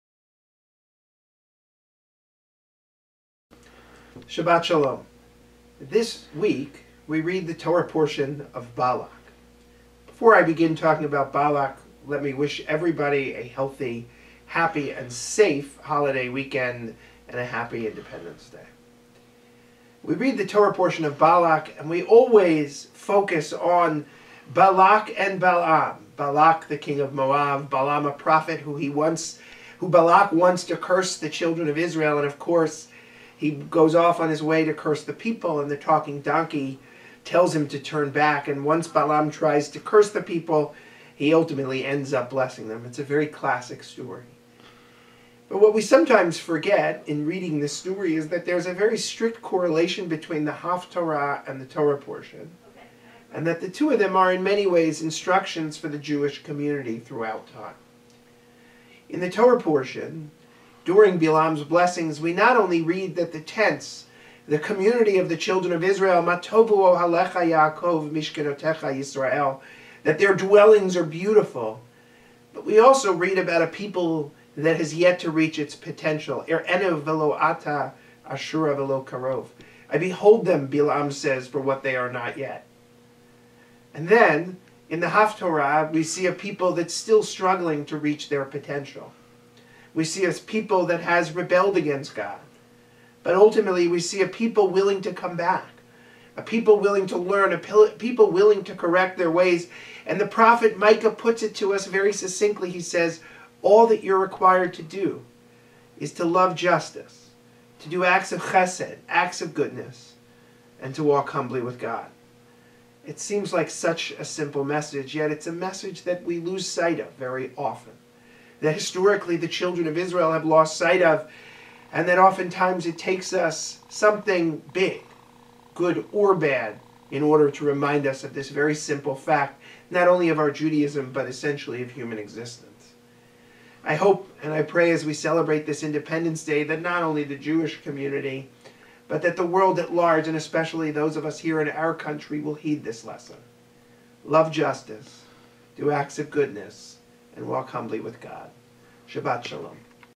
Rabbi Gruenberg's Weekly Message July 3 2015
Rabbi Joshua Gruenberg of Congregation Beth El of Bucks County delivers his weekly Torah commentary